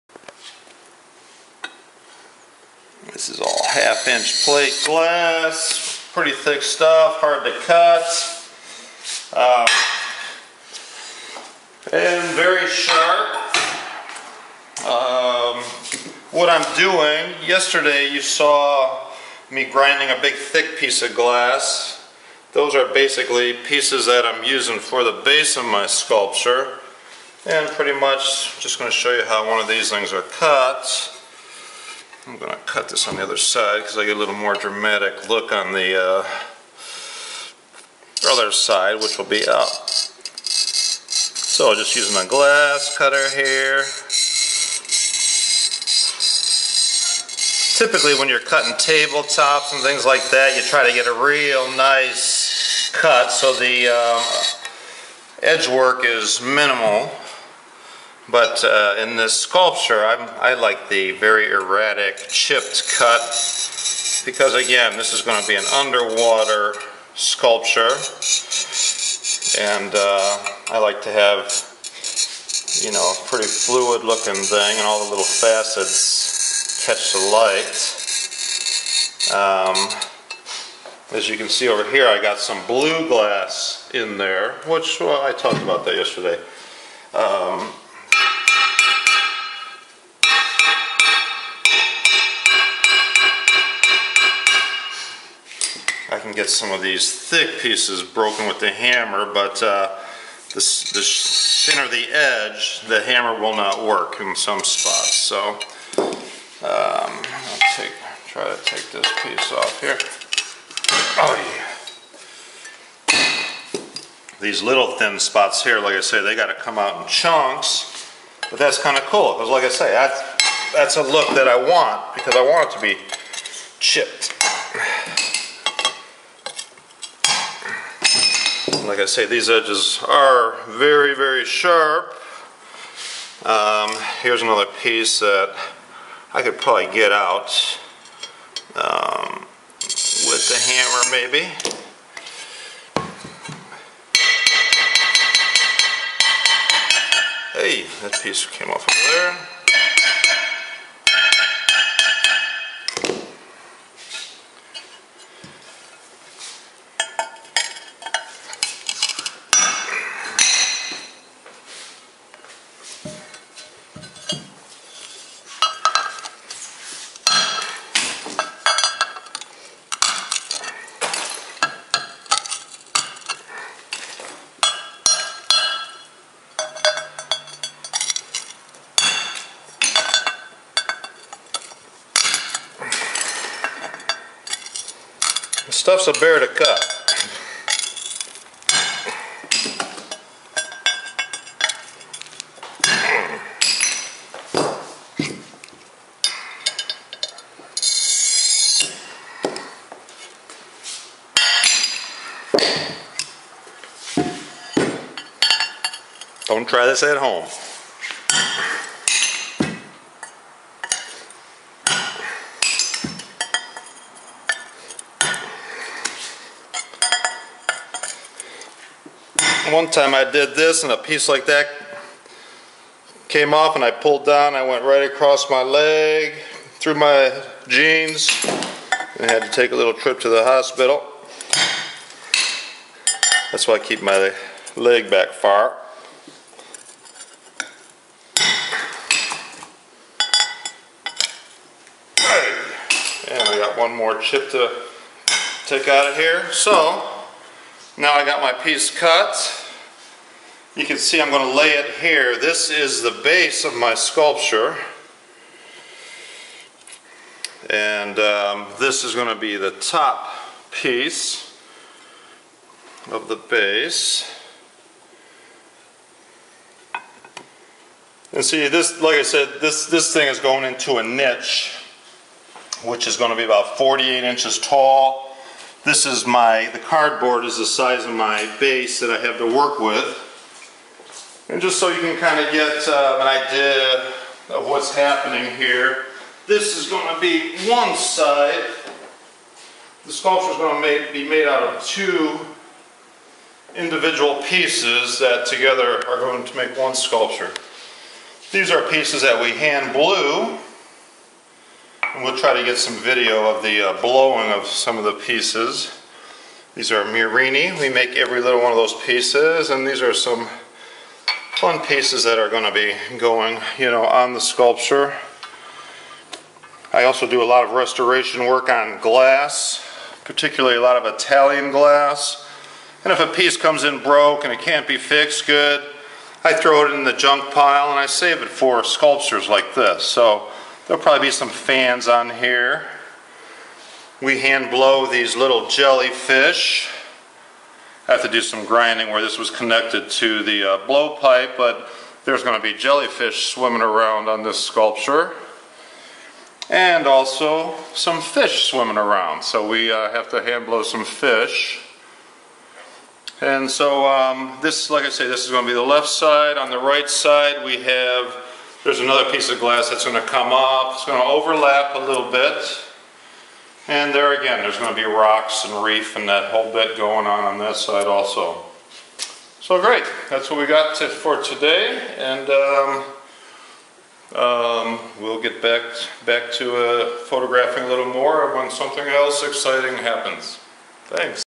Marblehead Ohio Master Glass Cutter Cary Ferguson Part 3
Here formerly of Cleveland, Ohio now sole proprietor of Ferguson Gallery now has his own gallery in Marblehead, Ohio creating an amazing sculpture. He studied under Walter Lagan for 17 years who consequently studied under world renowned Edward Drummond Libbey on numerous occasions.  Mr. Cary S. Ferguson is a master glass restorer, glass blower, and creates art glass marbles, and vases.